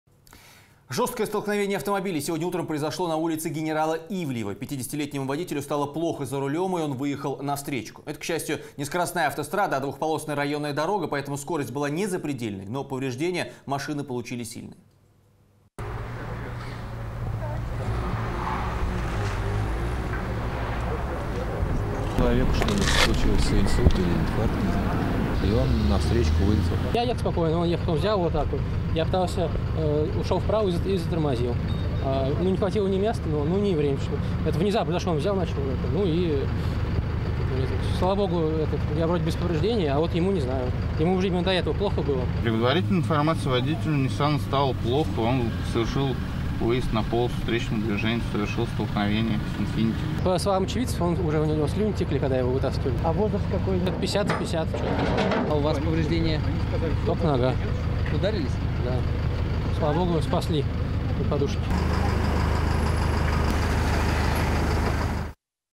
Водитель, которому стало плохо в машине, выехал на встречную полосу и совершил столкновение
Жесткое столкновение автомобилей сегодня утром произошло на улице генерала Ивлеева. 50-летнему водителю стало плохо за рулем и он выехал на «встречку». Это, к счастью, не скоростная автострада, а двухполосная районная дорога, поэтому скорость была не запредельной, но повреждения машины получили сильные.